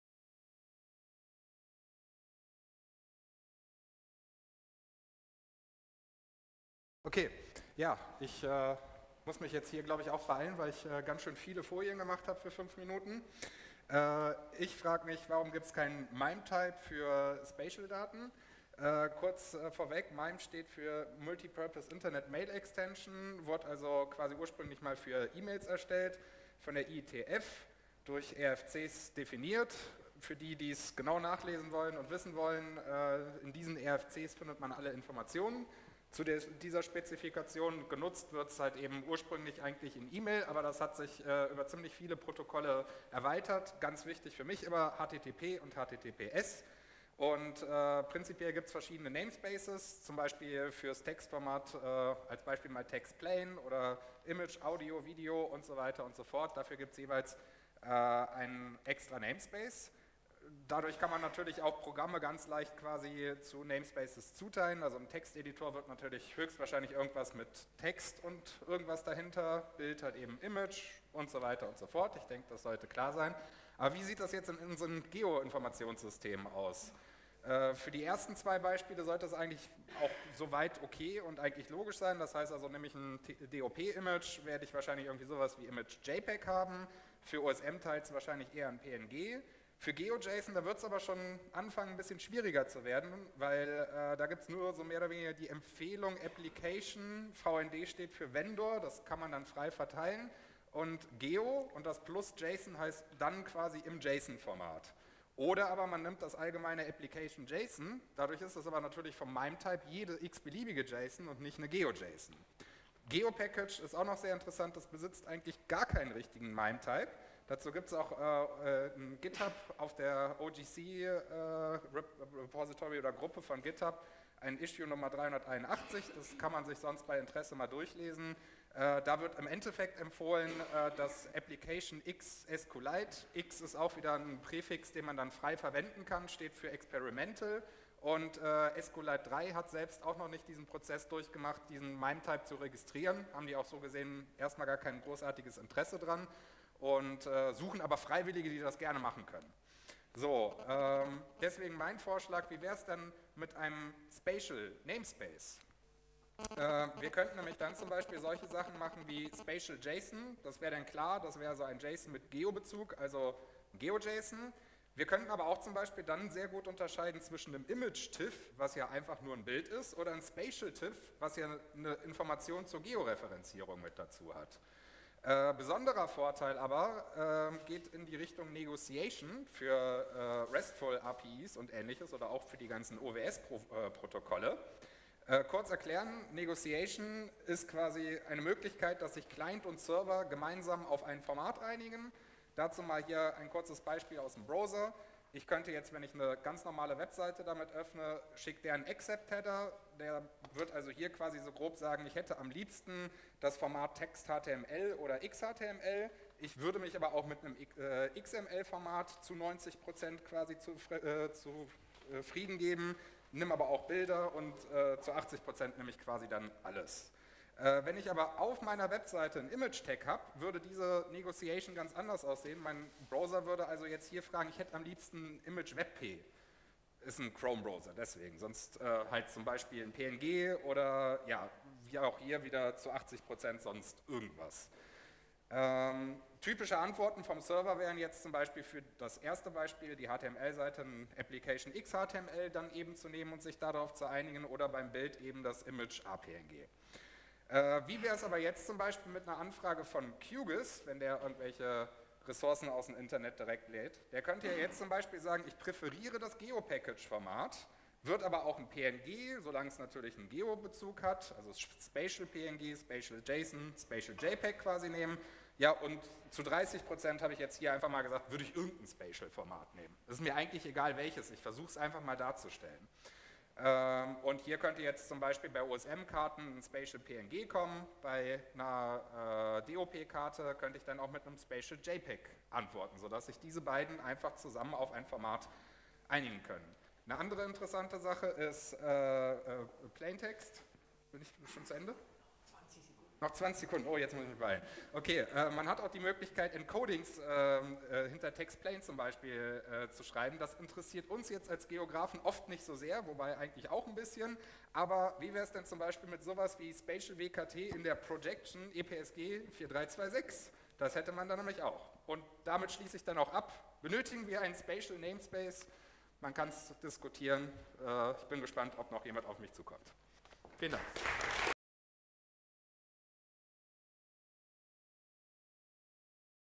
2018 - Why is spatial not mime?
Ein Lightning-Talk über einen MIME Namespace für Geodaten

Ein Vortrag über die Vorteile einer Einführung eines MIME Namespaces für Geo-Daten. MIME Types werden dafür eingesetzt Inhalte in Daten identifizieren zu können. Es gibt "type-extensions" für Audio, Video, Bilder und Text. Ich stelle die Frage: Wieso nicht für Geo-Daten?

In dem Vortrag möchte ich ein Thema anschneiden, über das ich mich zuvor auf mehreren Veranstaltungen mit einzelnen Personen bereits unterhalten hatte, aber hier dem Thema ein größeres Forum geben will:

Wieso gibt es eigenltich kein MIME Namespace für Geo-Daten?

In dem Lightning-Talk soll aufgezeigt werden:
- welche Bedeutung und Vorteile dies für viele Geo-Standards hätte
- das dies dazu genutzt werden könnte die bekannten OGC Standards zu modernisieren
- das der Weg hier hin gar nicht so schwierig zu sein scheint.

Es muss scheinbar nur in Angriff genommen werden!

Arne Schubert